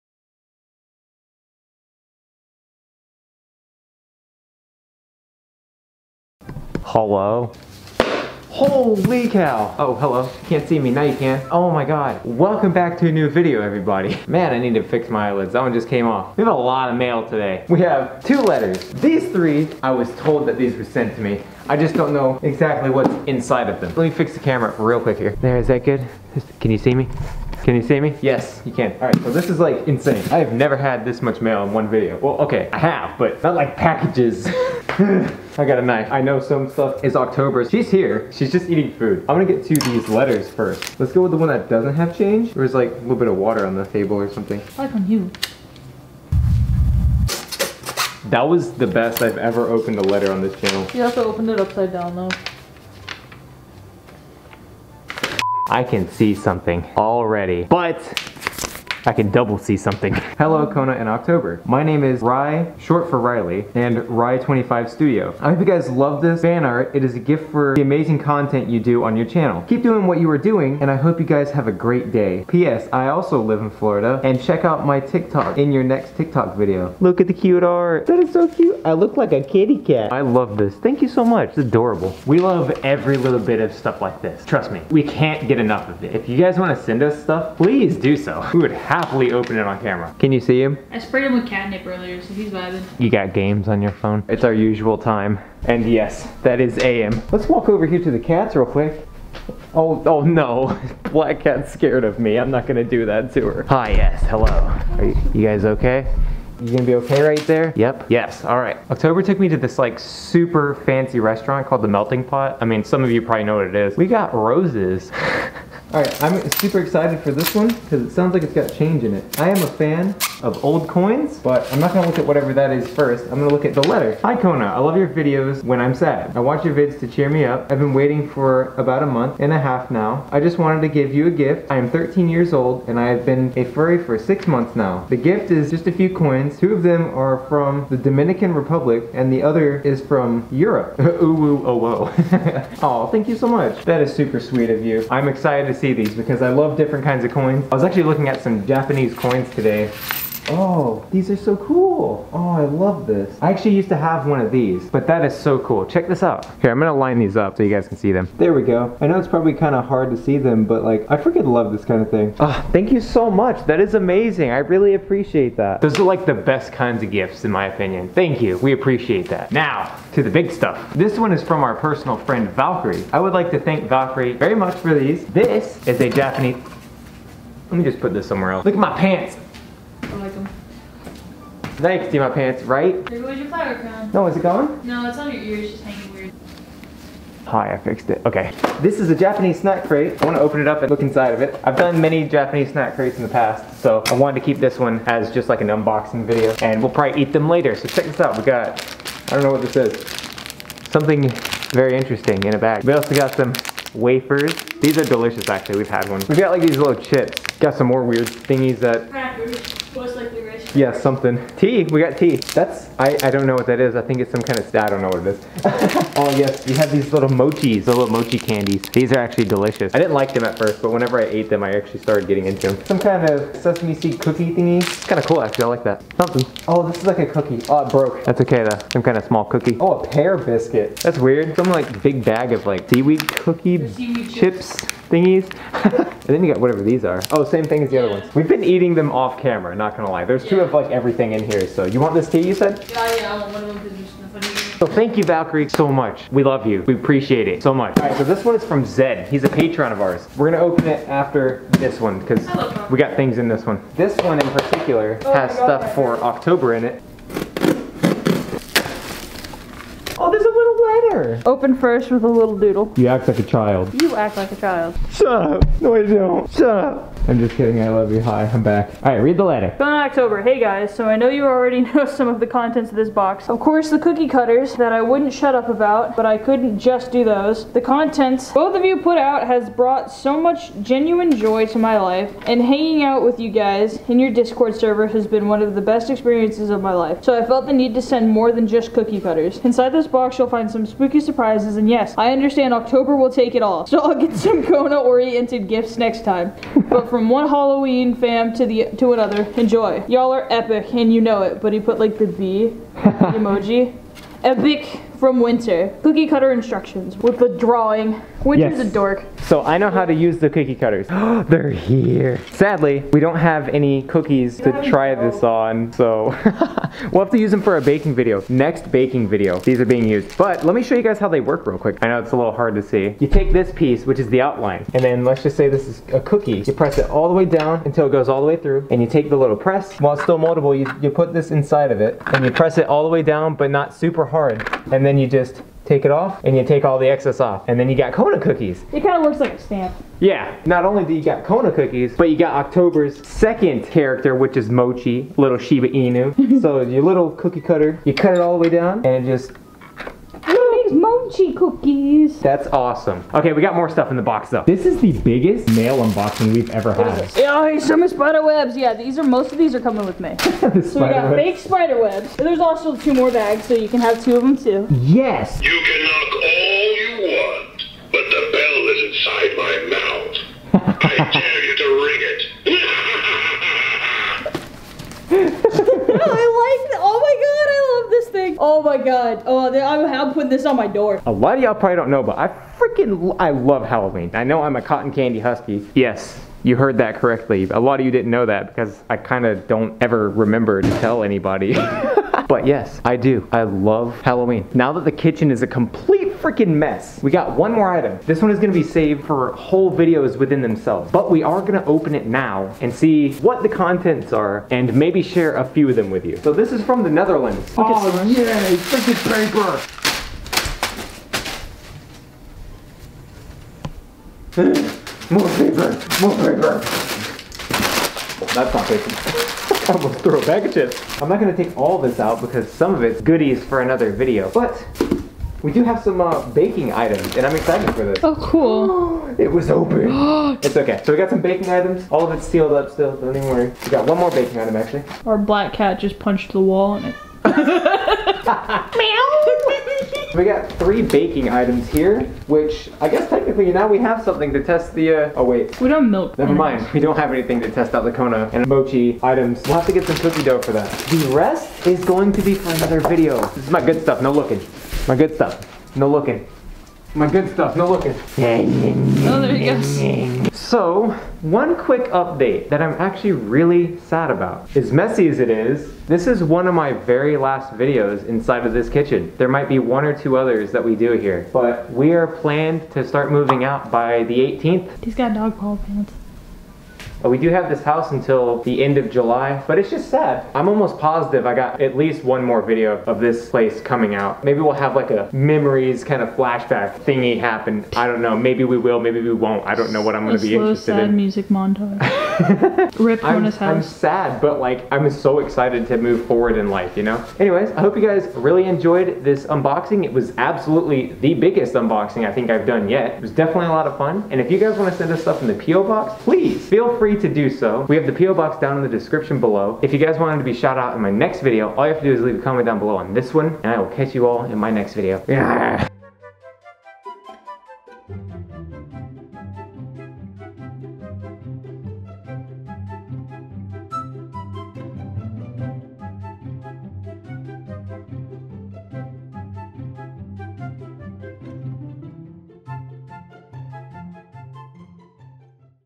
Biggest Mail Unboxing Yet! (Fan Mail unboxing #5)
I am beyond happy with absolutely everything you all send us, we love and appreciate every one of you so much!!

Camera Used

Thank you to Luxo Fox For allowing us to use the art provided in the outro!